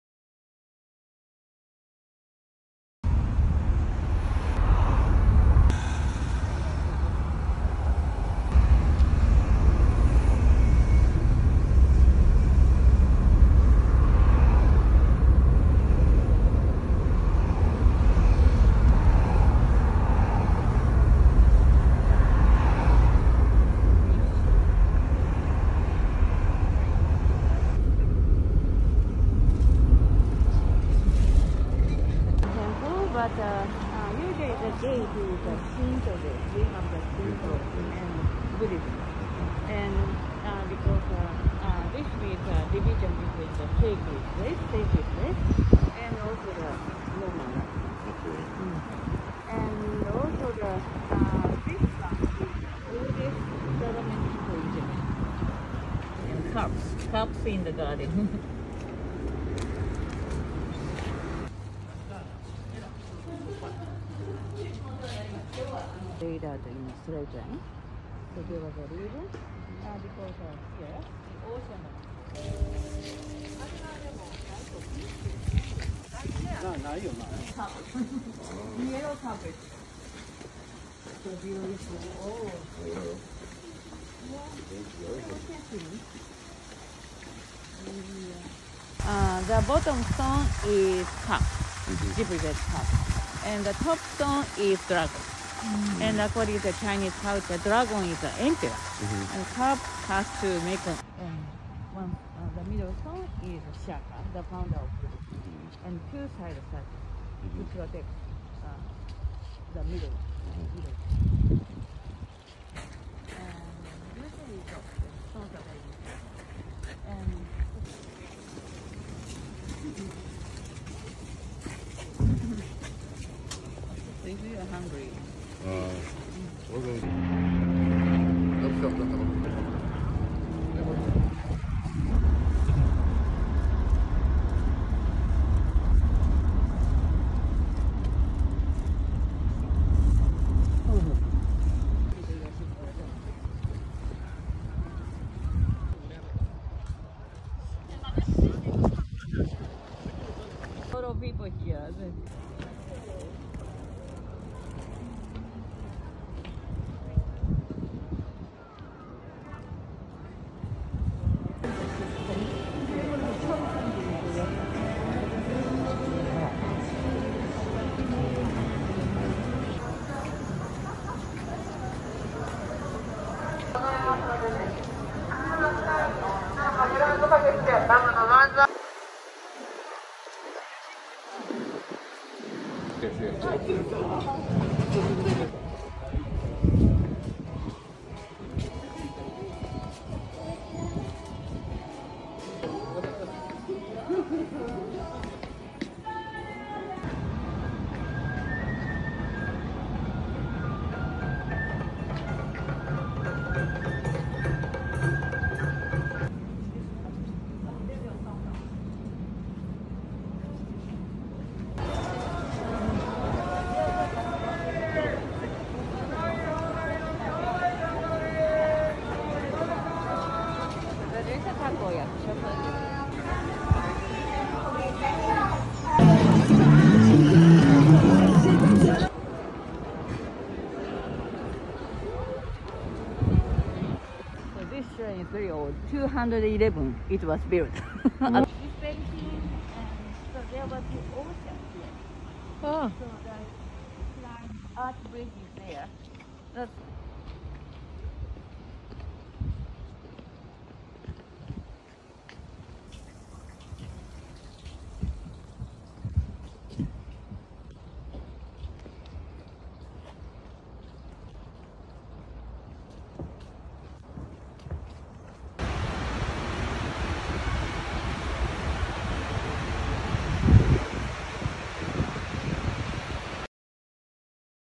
Osaka December 2022 ASMR
Tour Osaka by car in December 2022.